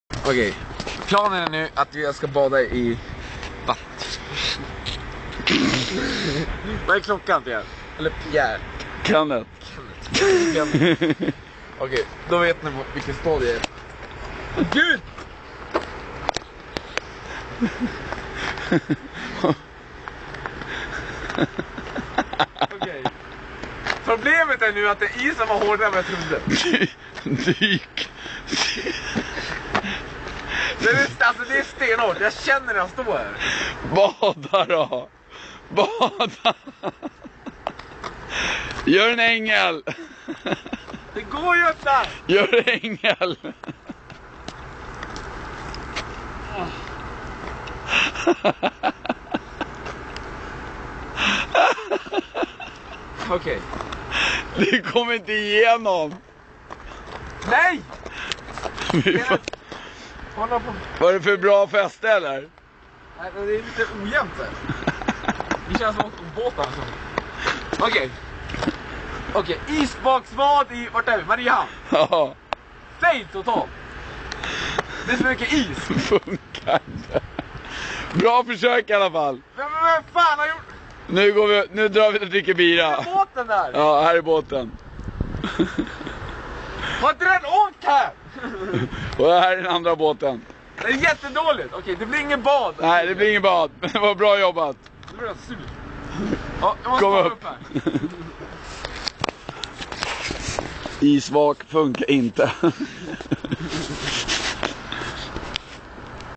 Greenman Mariehamn
Greenman försöker bada i Mariehamn